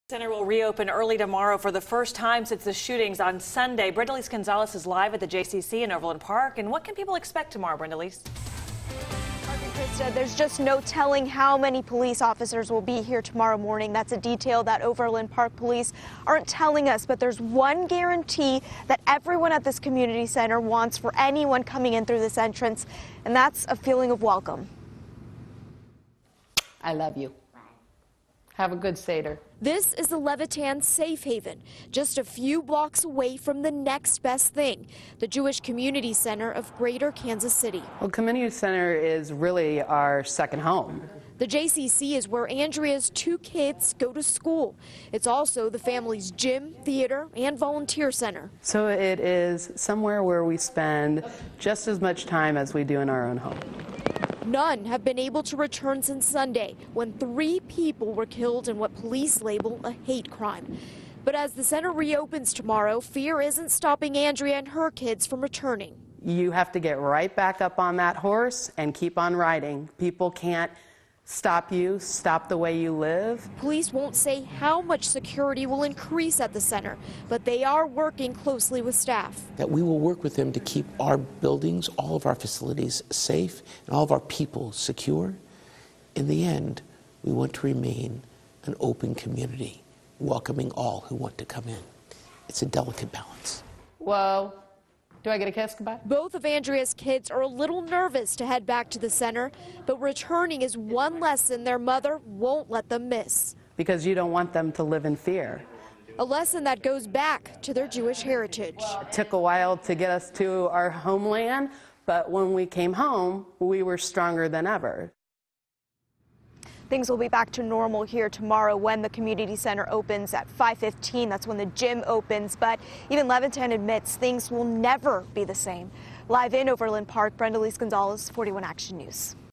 Jewish Community Center reopens Wednesday morning
The center will reopen early Wednesday for the first time since Sunday's shootings.